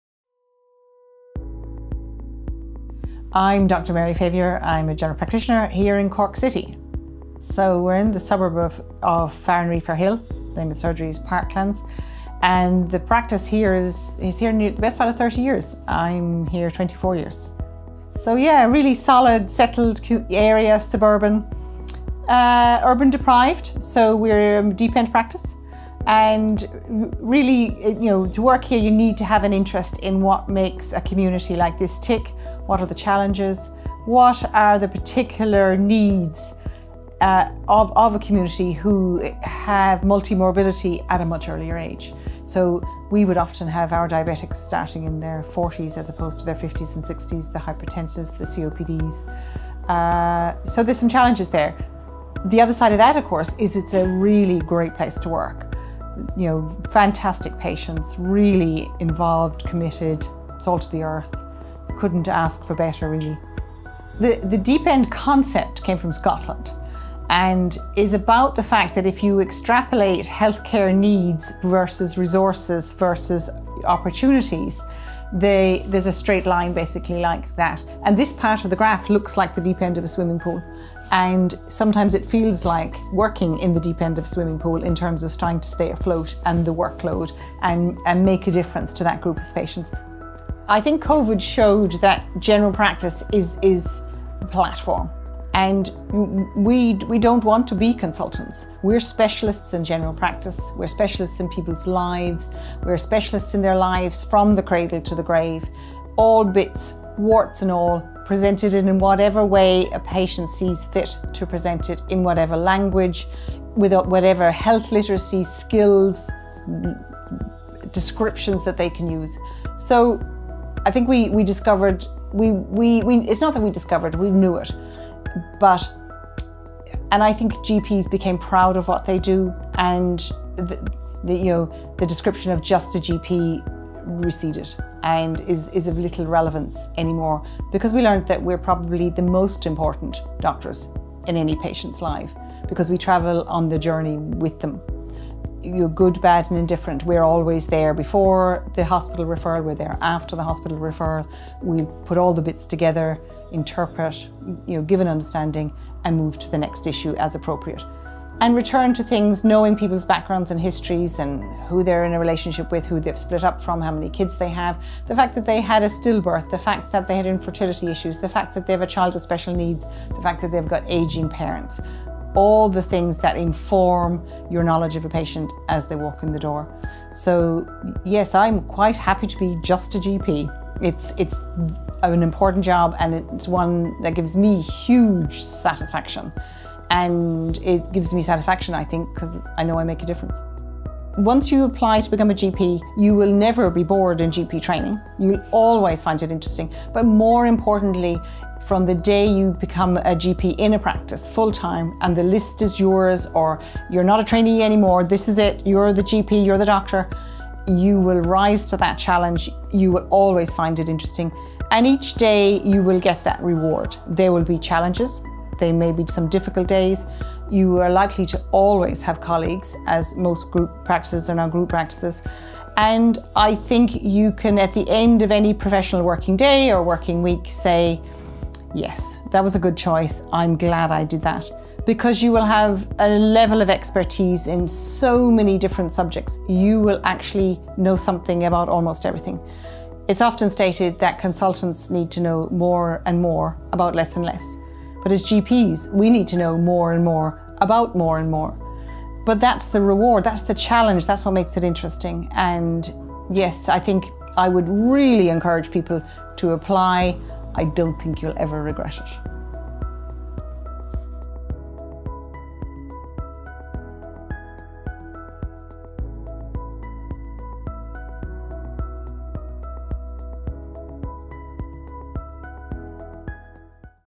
BEAGP - Dr Mary Favier
Do you want to make a difference in people’s lives, in communities facing challenges? Being a GP in a lower-income community is a special role.
Dr Mary Favier describes how her work makes a difference in Cork city.

Applications for the 2022 GP Training scheme open on 13th October 2021 and close on 17th November 2021.